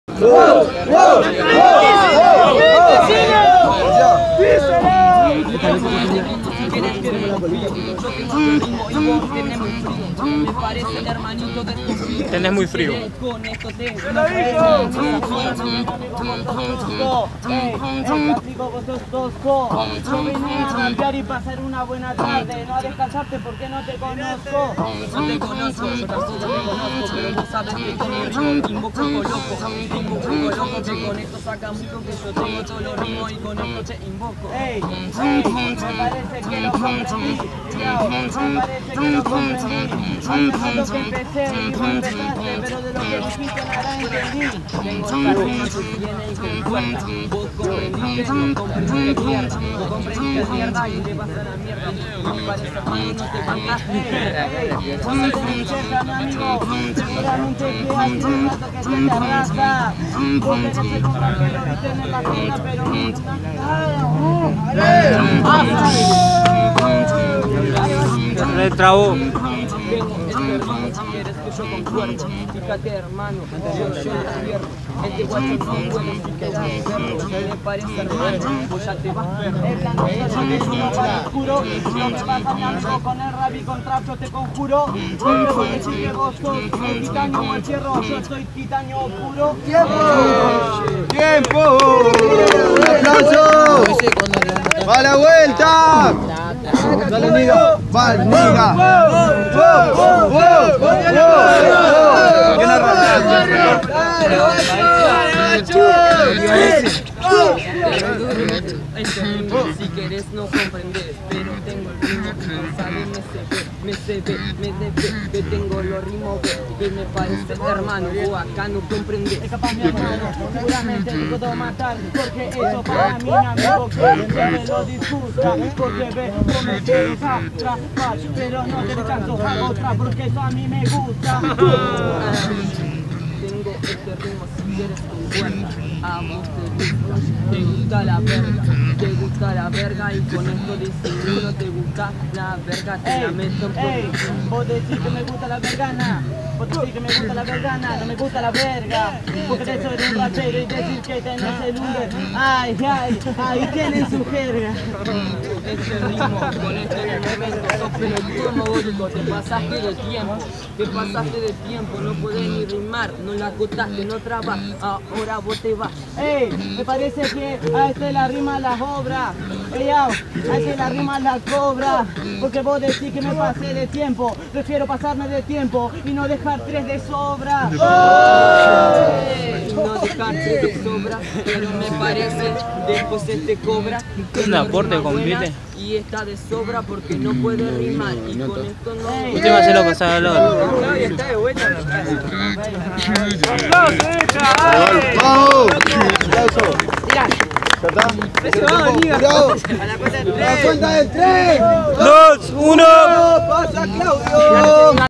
Claudio vs Dabeat 8vo Warrior del Under 3era Edicion
Competencia: Warrior del Under

Modalidad: 1 vs 1 (4 x 4)

Lugar: Parana, Entre Rio - Plaza de mayo

Ganador: Claudio